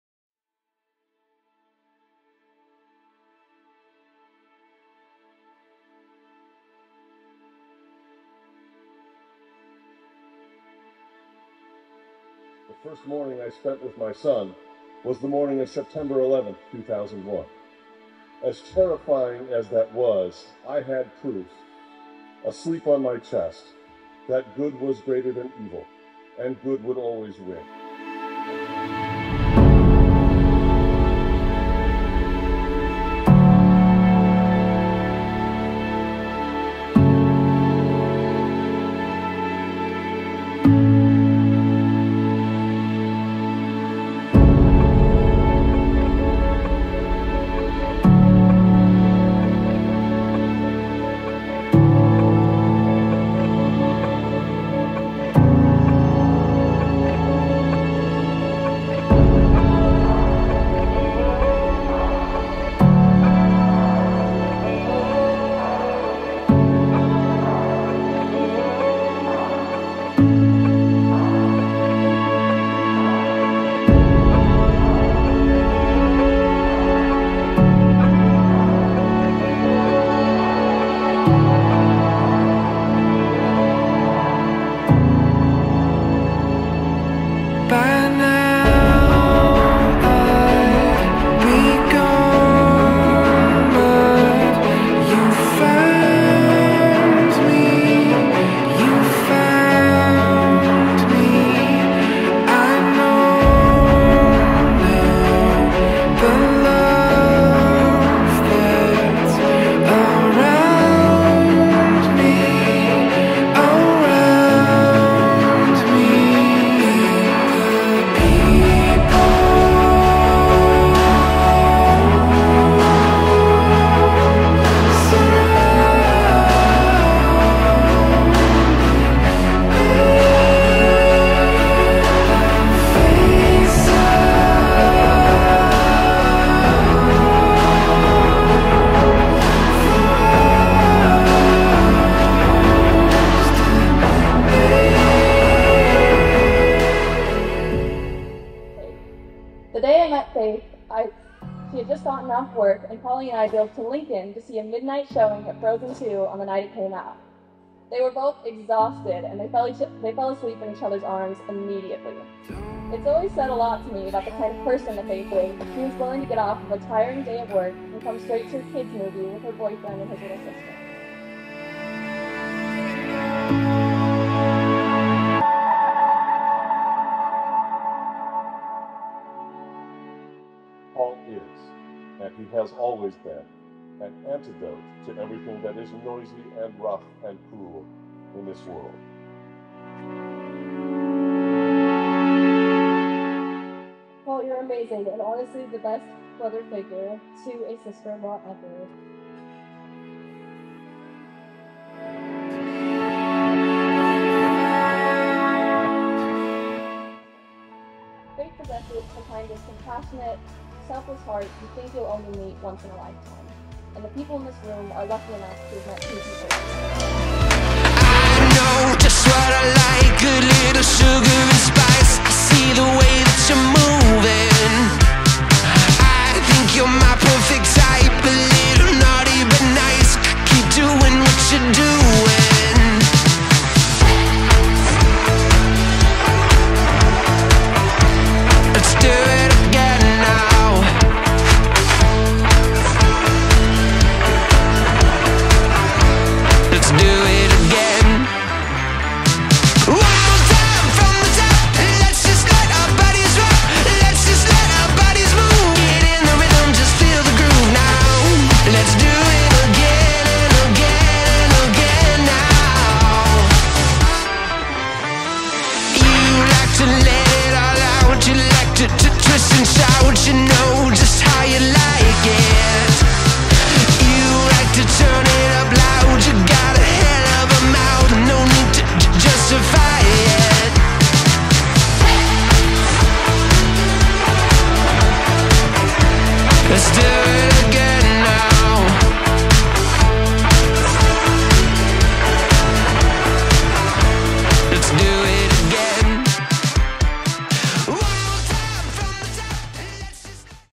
Faith and Paul's Wedding Film | Fountains Ballroom, IA
The epitome of young and in love. Faith and Paul have a special relationship that is full of fun, passion, and quirkiness. This day was nothing short of magic and full of their family and friends that helped them celebrate their love so well.